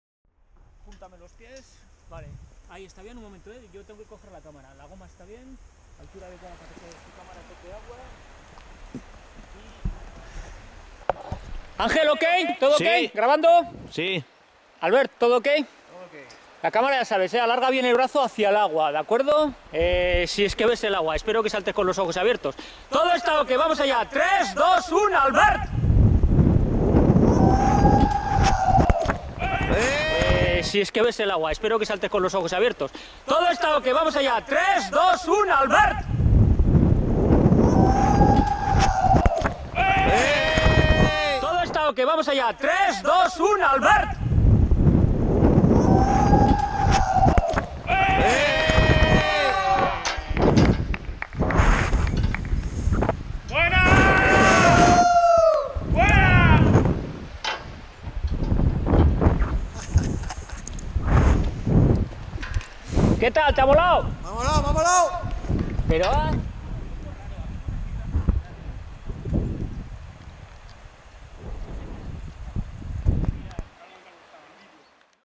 góming, bungee jumping, gomasaltoa, saut à l'élastique Palháis ACE2
El puente de Palháis es pequeñito pero simpático, nos permite hacer puénting sobre agua (sin contacto, claro), góming tocando agua y lo estamos preparando para que nos permita hacer pupuénting (supercerquita del agua, pero sin tocarla, por favor) y pénduling tocando agua.
Además, para los cursos de formación resulta, mucha veces, más adecuado, además de por todo lo anterior, por su tranquilidad, accesibilidad y porque tiene, muchas, veces, las características más parecidas a las de los que van a emplear nuestros cliente.